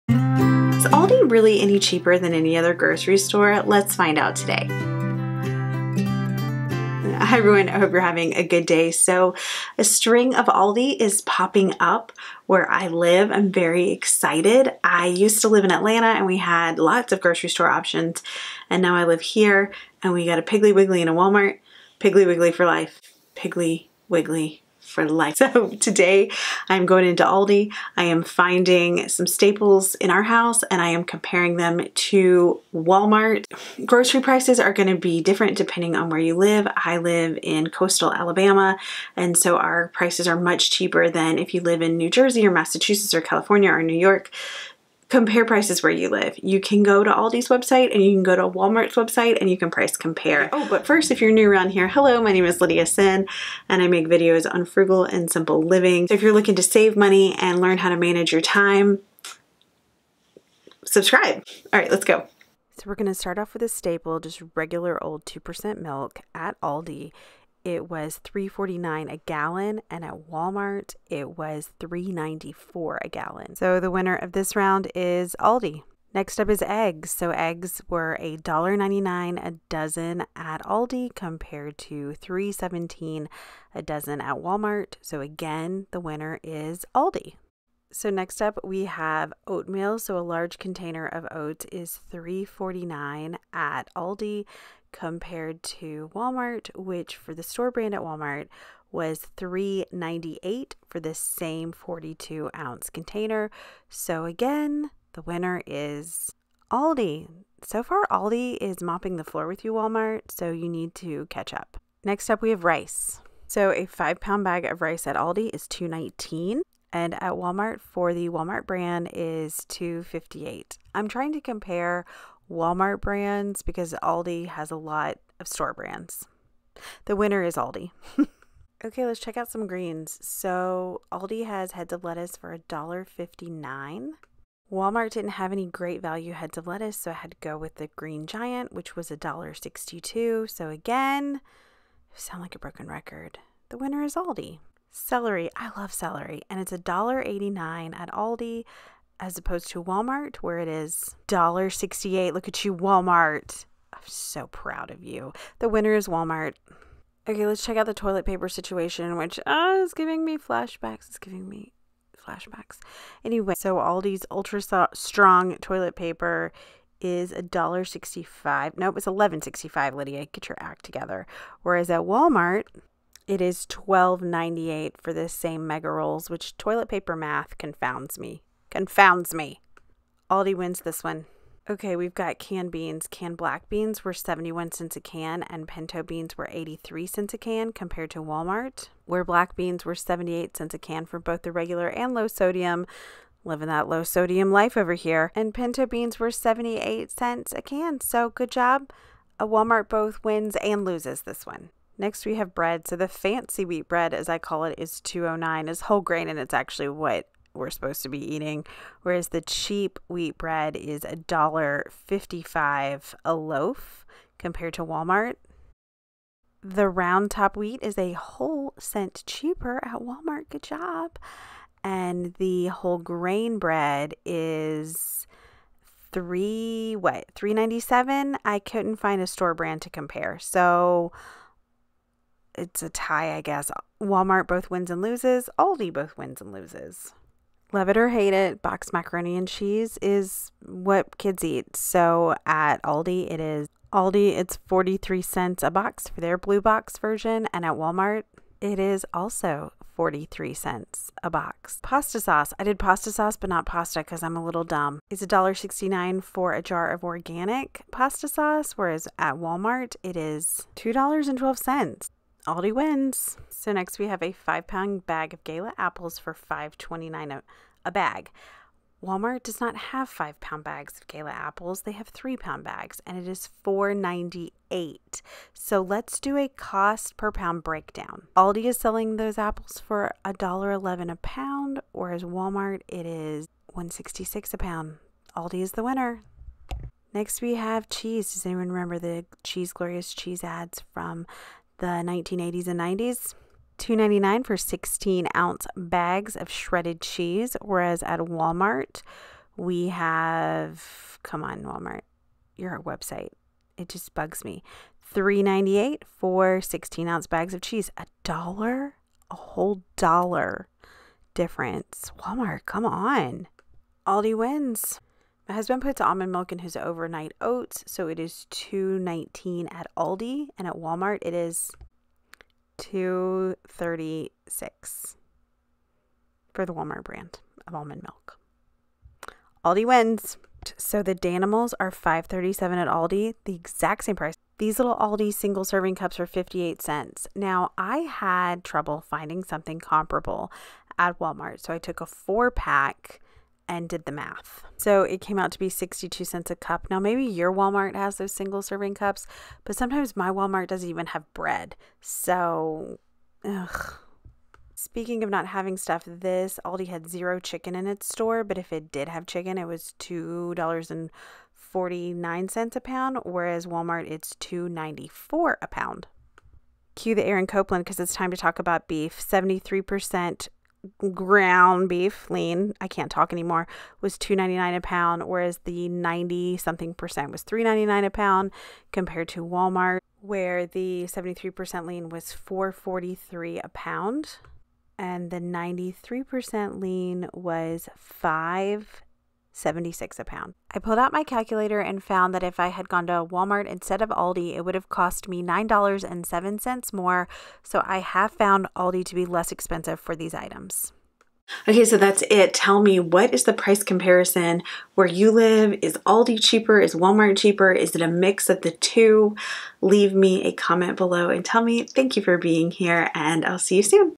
Which is cheaper? Comparing Aldi to Walmart
Today I am comparing which is cheaper, Aldi or Walmart.

Snail Mail:
Lydia Senn
Creola, AL 36525